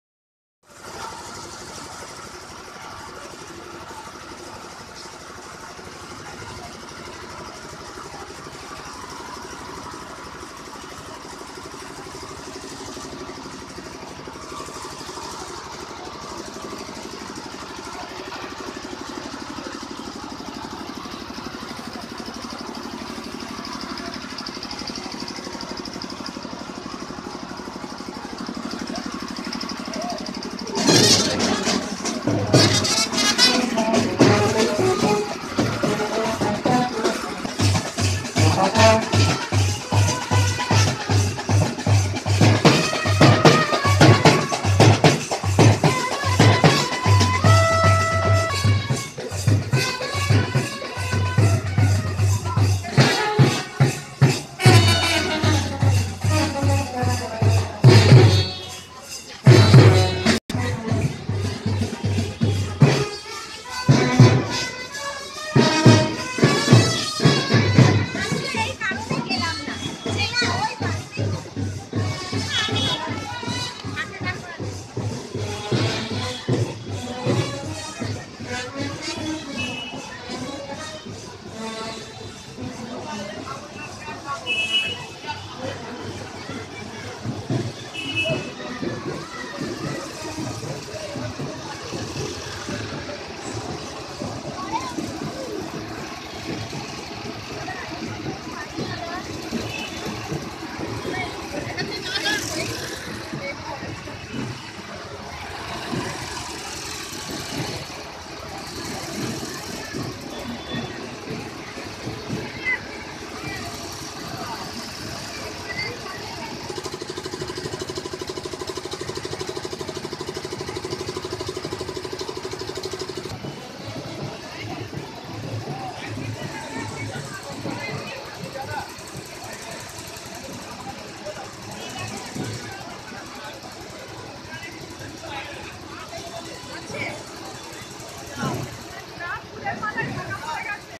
নদিয়া ডিসটিক কৃষ্ণনগর এর মা কালীর বিদায় যাত্রা, প্লিজ সাবস্ক্রাইব,
কৃষ্ণনগর,
কালীপূজা
ব্যান্ড পার্টি নিয়ে বিসর্জন যাত্রা,